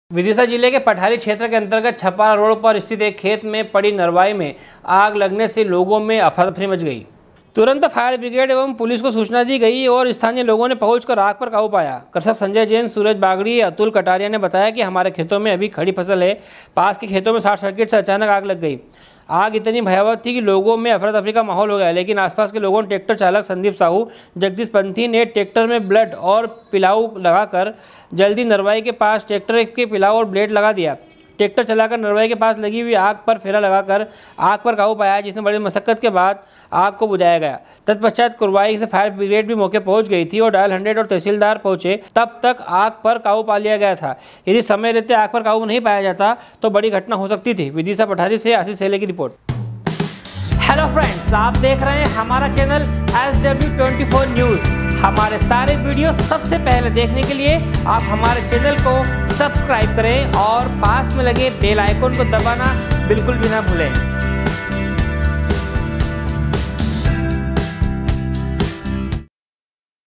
vidisha- विदिशा जिले के पठारी क्षेत्र के अंतर्गत छपारा रोड पर स्थित एक खेत में खड़ी नरवाई में
विदिशा जिले के पठारी  क्षेत्र के अंतर्गत छपारा रोड पर स्थित एक खेत में खड़ी नरवाई में आग लगने से लोगों मे अफरा-तफरी मचगई। विदिशा पठारी से आशीष सहेले की रिपोर्ट